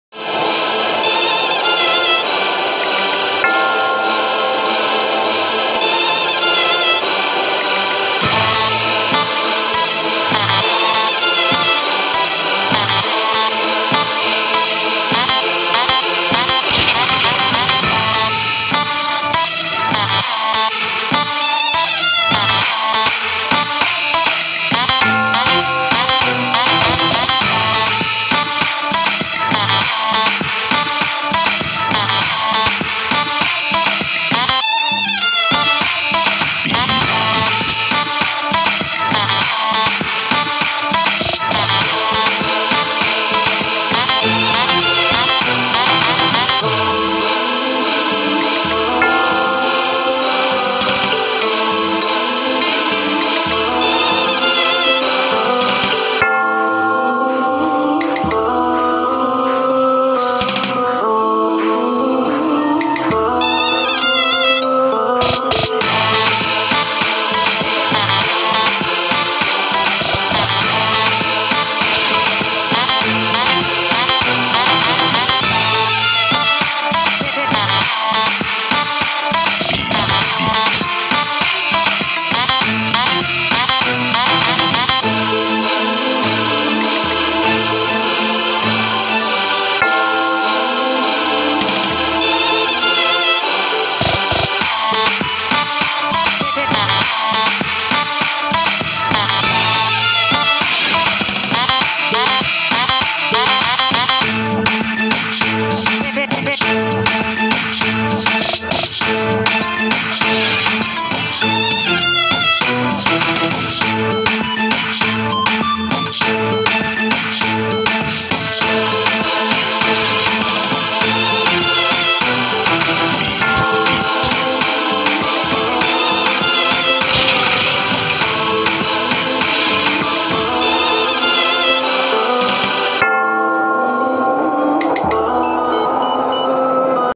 Urbankiz Dance
Another kizomba practise. when lock down 2years ago